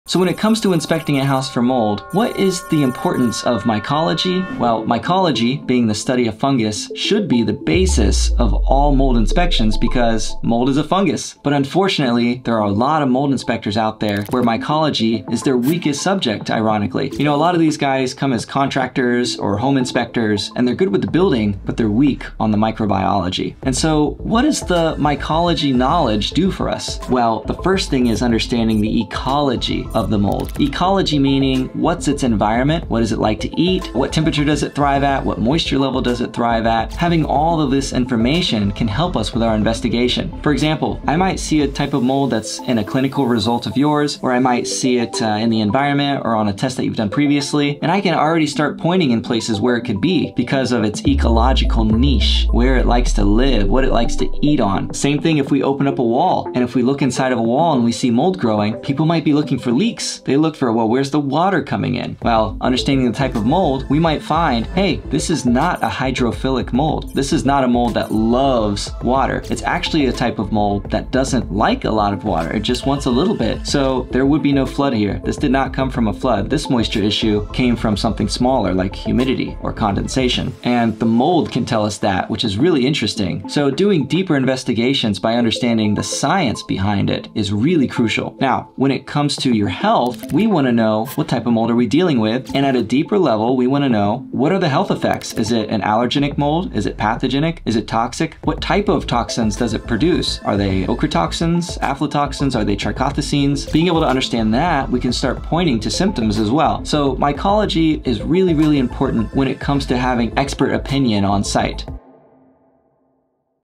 Mycology? Your Mold Inspector MUST HAVE This Knowledge!
Mycology is the study of fungi! Without at least the fundamentals of mycology, your mold inspector may not be able to provide expert opinion. It is one thing to memorize the name of three or four toxic molds and another thing to truly understand them.

Purchase our number one recommended air purifier here!